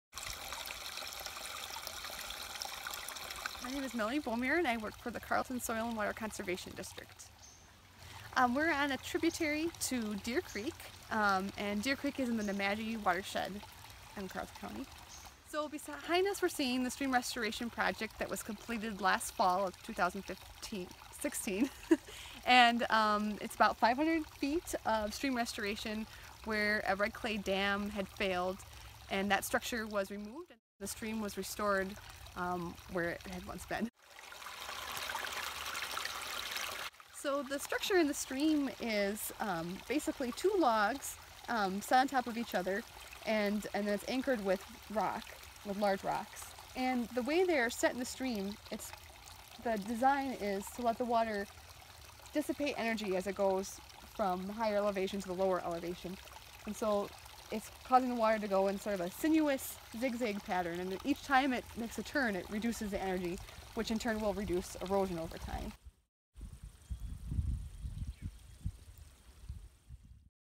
Red Clay Dam Fix
Engineers positioned every boulder, every log to curb velocity and direct the flow of rushing water.

You won’t notice the engineering in this remeandered stream once grasses, shrubs and trees grow up through the straw and coconut-fiber mats on this Carlton County hillside. A Clean Water Fund grant helped finance the project. Here, a failed red clay dam released 30 years’ accumulated sediment into the Nemadji River and Lake Superior.